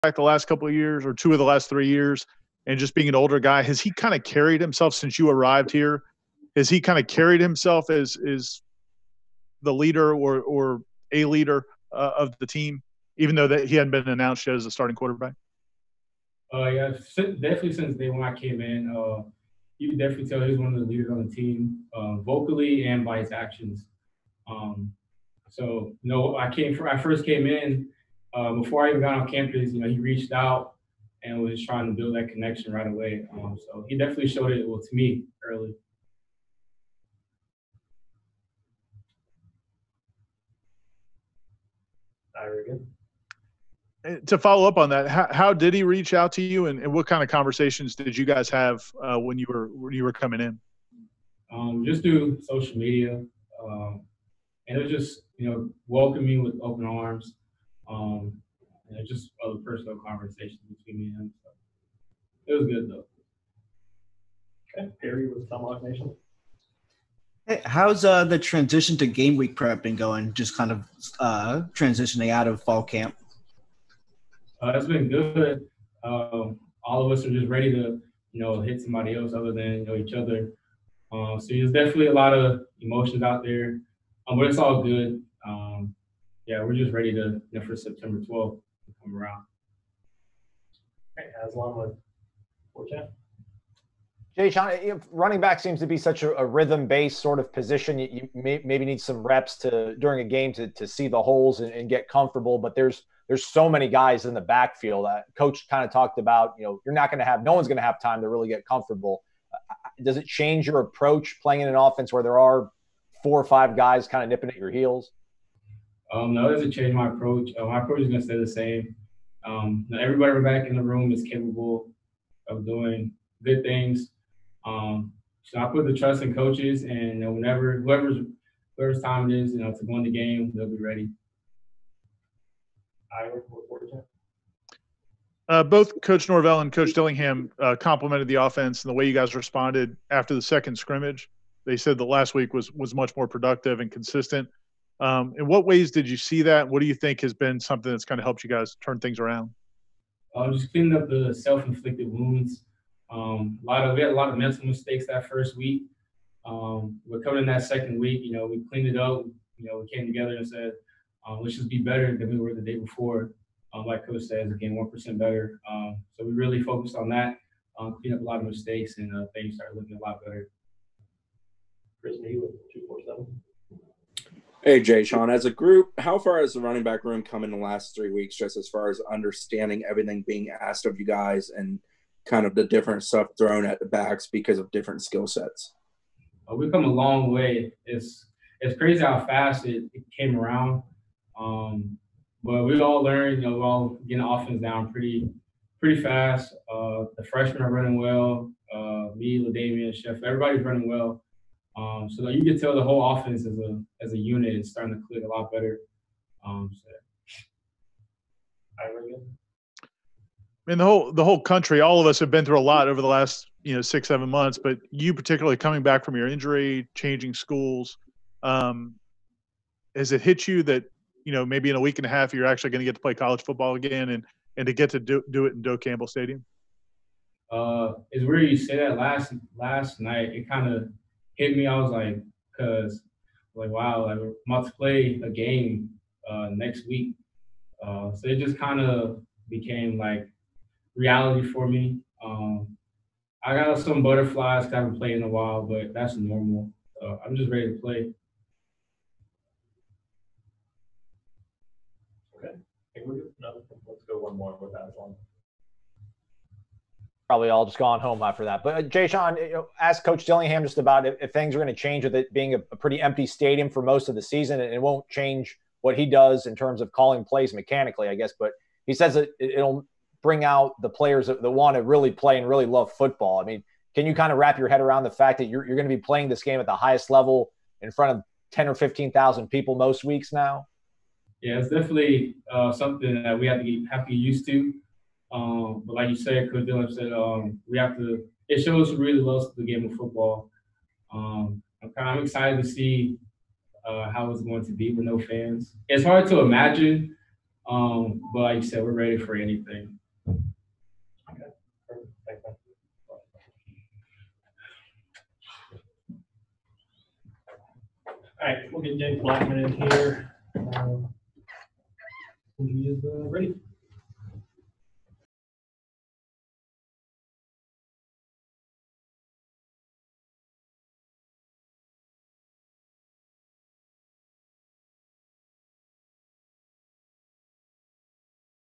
FSU RB Jashaun Corbin on offensive growth, Blackman earning start
Corbin sees continued progress from the offense